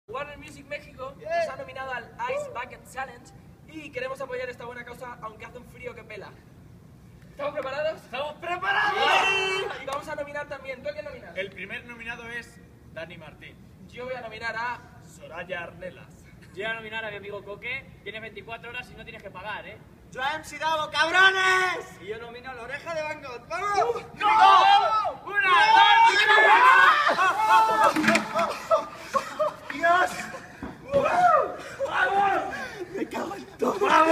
Auryn - ALSIceBuckedChallenge
¡Auryn cumplió el reto que Warner Music México dictó!

Sus nominados fueron MC DAVO, La Oreja de Van Gogh, Dany Martín, Soraya Arnelas y Koke.